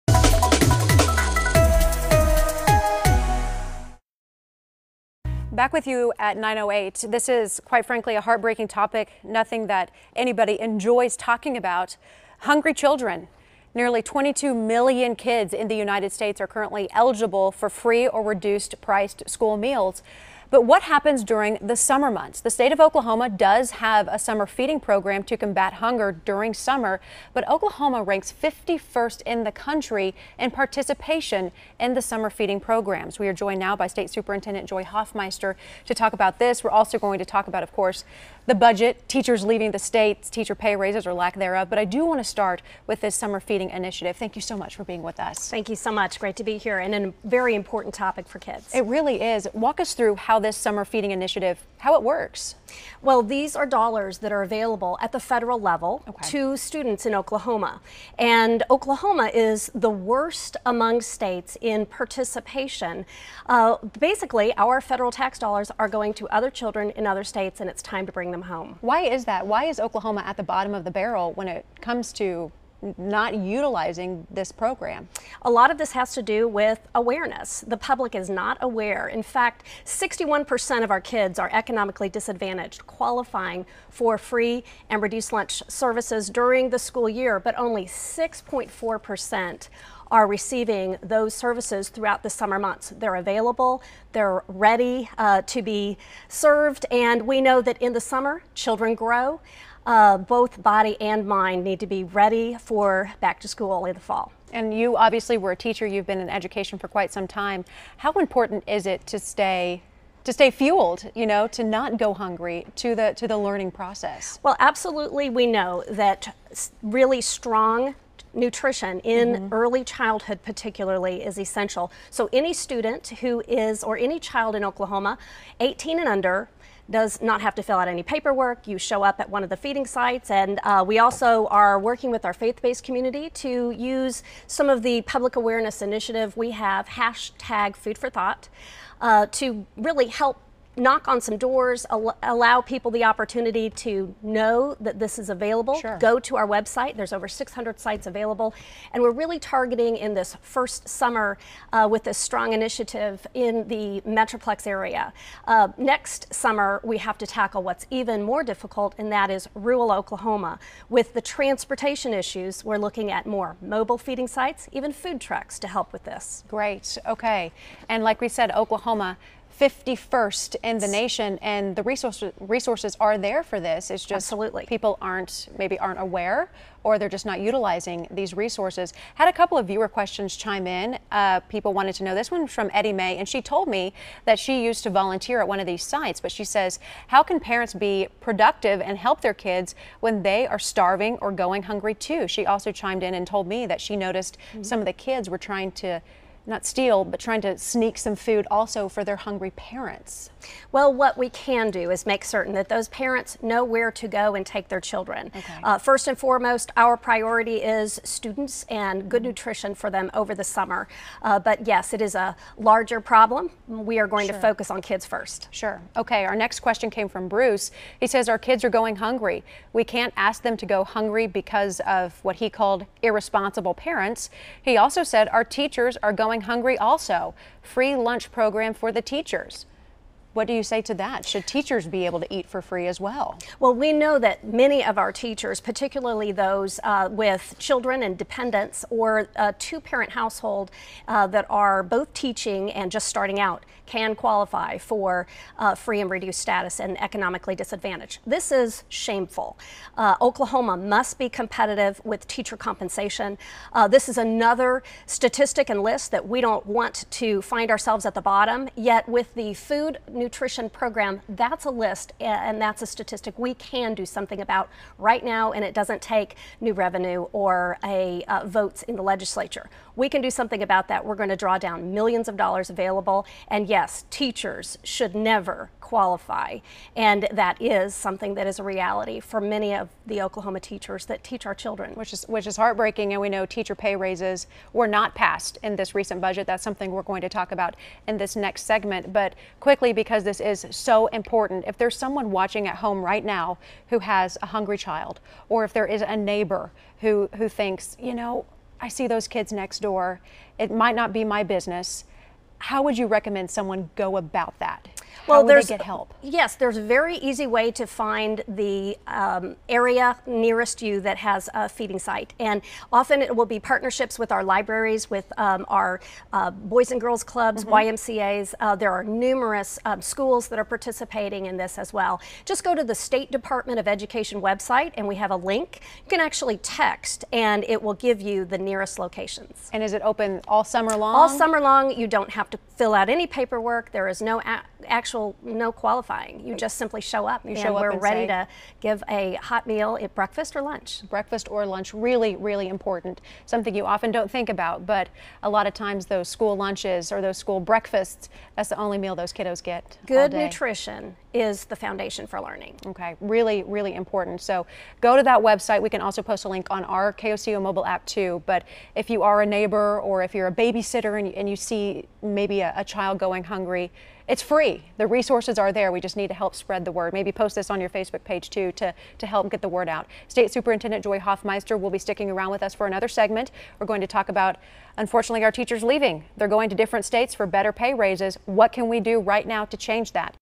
Okla. Superintendent Joy Hofmeister talks about summer feeding programs
The State of Oklahoma has a summer feeding program to combat hunger during the summer. State Superintendent Joy Hofmeister joins KOCO 5 this morning to talk about the summer feeding programs, as well as teachers leaving for other states because of low pay.